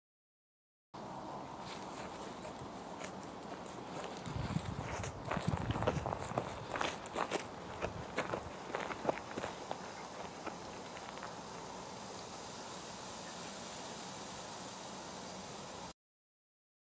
Gimbal Zhiyun Weebill S. Test 1 (Camera Panasonic GH4, 12-35/2.8 I)
My first attempt to use a gimbal.